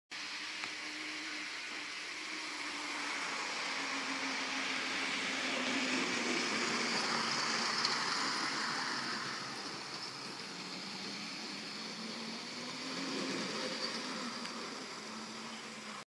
Model steam train running round in the Toy collector museam in Waltham
This is a model steam train running round on a display inside the Toy collector museam. If you want to pay a visit to that museam its on 90 Falsgrave street, Waltham, Christchurch 8011 and it costs $10.00 per person to get in. The displays there are so cool and it is worth a visit there.